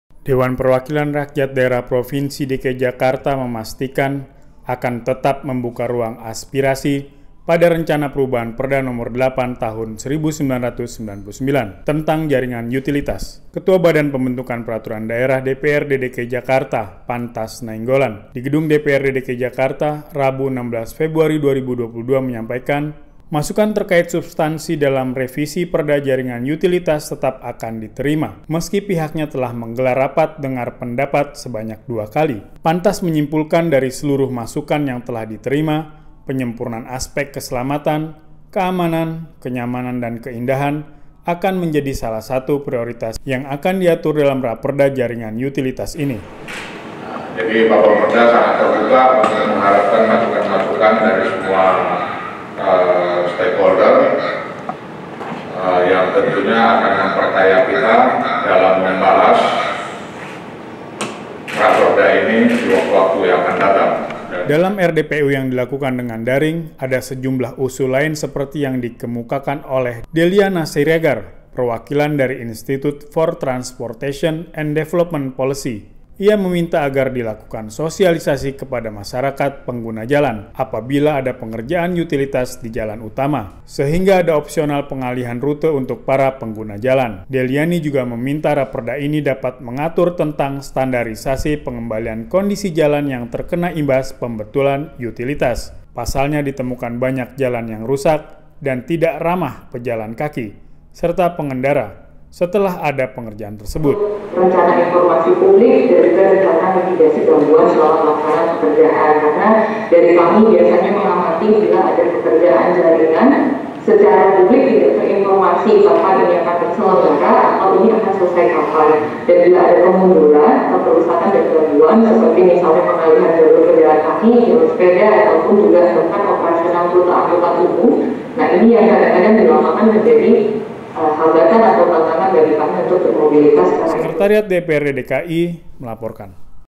DPRD Tetap Buka Ruang Aspirasi di Revisi Perda Jaringan Utilitas
Dewan Perwakilan Rakyat Daerah (DPRD) Provinsi DKI Jakarta memastikan akan tetap membuka ruang aspirasi pada rencana perubahan Perda Nomor 8 Tahun 1999 tentang Jaringan Utilitas.

Ketua Badan Pembentukan Peraturan Daerah (Bapemperda) DPRD DKI Jakarta Pantas Nainggolan menyampaikan, masukan terkait subtansi dalam revisi Perda Jaringan Utilitas tetap akan diterima meski pihaknya telah menggelar rapat dengar pendapat (RDP) sebanyak dua kali.